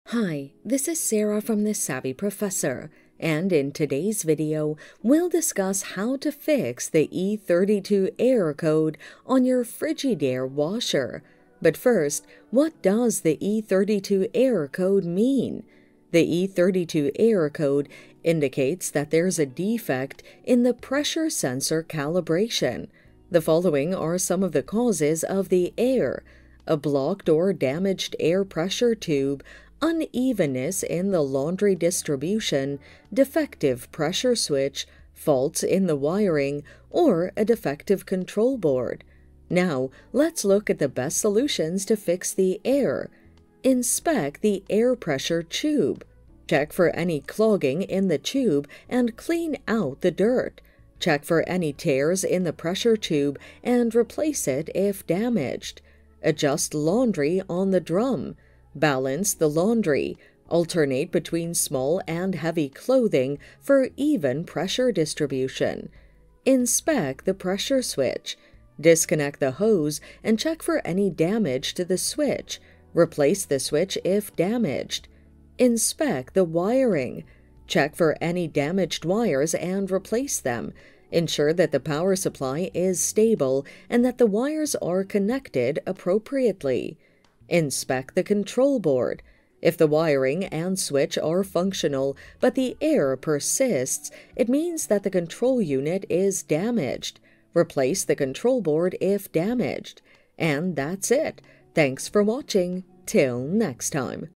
How To Fix Frigidaire Washer Error Code E32 (Understand The Causes Of Error E32 - Simple Solutions!)
How To Fix Frigidaire Washer Error Code E32 (Understand The Causes Of Error E32 - Simple Solutions!). In this video tutorial I will show you how to fix Frigidaire Washer error code E32.

The e32 code indicates that there is a defect in the pressure sensor calibration.

The following are some of the Causes of the Error:
• Blocked or damaged air pressure tube.
• Unevenness in the laundry distribution
• Defective pressure switch
• Faults in the wiring
• Defective control board

Now, Let’s Look at the Best Solutions to Fix the Error:
Inspect the air pressure tube
• Check for any clogging in the tube and clean out the dirt.
• Check for any tears in the pressure tube and replace it if damaged.

Adjust laundry on the drum
• Balance the laundry.
• Alternate between small and heavy clothing for even pressure distribution.

Inspect the pressure switch
• Disconnect the hose and check for any damage to the switch.
• Replace the switch if damaged

Inspect the wiring
• Check for any damaged wires and replace them.
• Ensure that the power supply is stable and that the wires are connected appropriately.

Inspect the control board.
• If the wiring and switch are functional, but the error persists, it means that the control unit is damaged.
• Replace the control board if damaged.